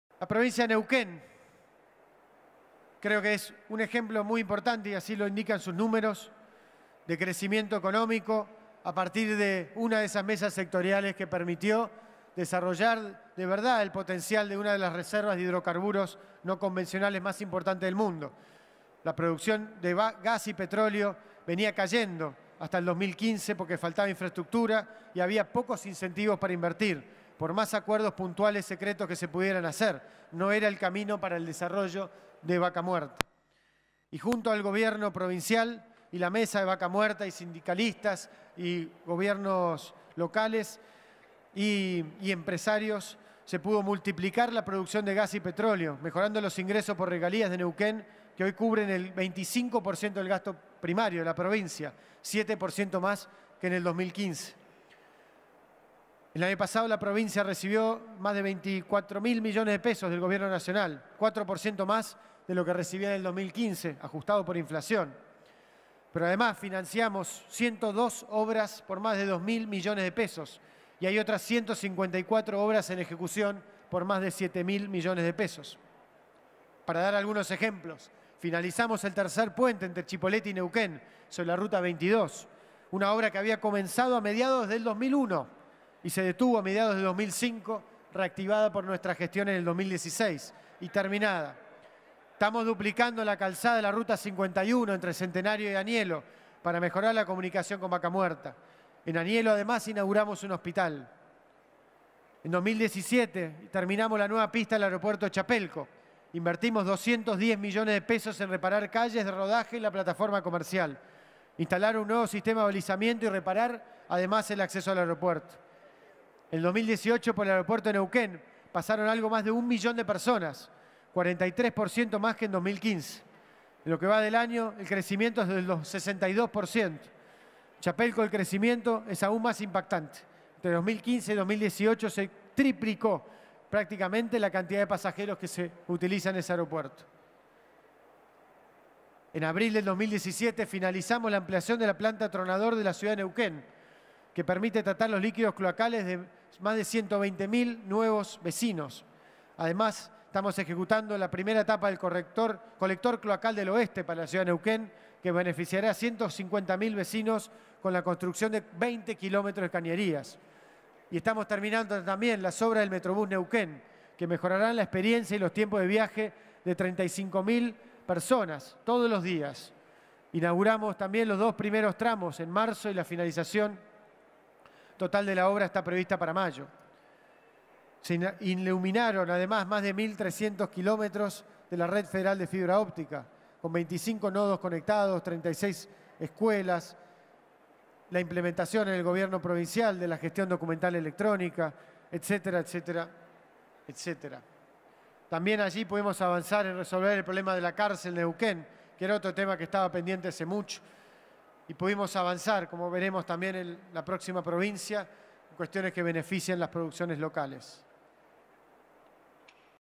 Marcos Peña en el Congreso - Neuquén Al Instante
El jefe de Gabinete Marcos Peña da su primer informe de gestión ante los diputados nacionales. La exposición se dividió en dos partes. Primero, el funcionario se dedicó a repasar la situación provincia por provincia, para luego brindar un resumen general de los temas de la gestión, incluyendo el tema económico. Hizo referencia a Neuquén, Vaca Muerta y la situación provincial.